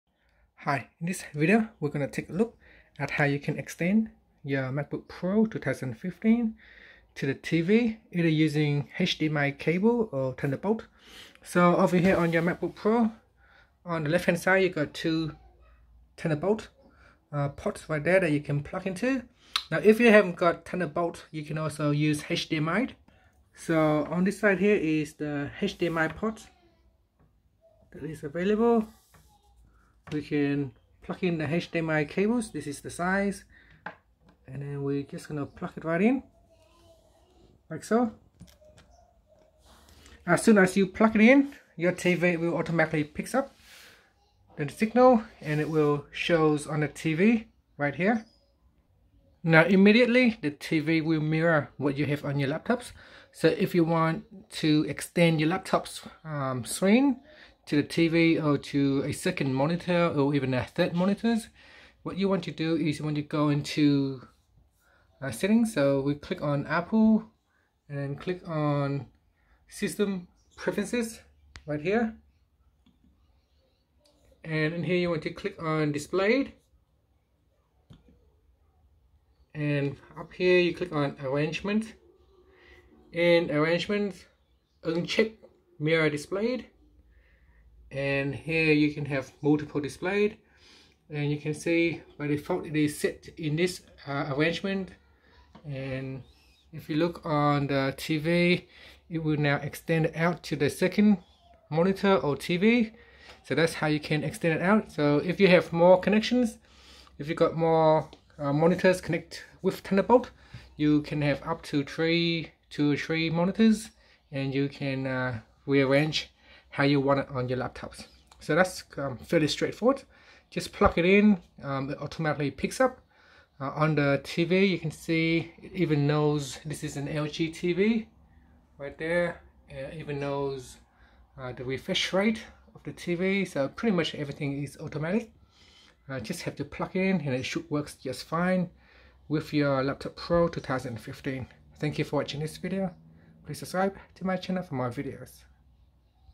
How to Extend MacBook Pro 2015 Screen To a TV or Multiple Monitors (HDMI / ThunderBolt)
Learn how you can extend or mirror the MacBook Pro 2015 screen to a TV or Multiple monitors. You can connect using the HDMI cable or the Apple thunderbolt 2 cable.

UGREEN Mini DisplayPort to HDMI VGA Adapter

AmazonBasics Mini DisplayPort to HDMI Display Adapter Cable

AmazonBasics High-Speed HDMI Cable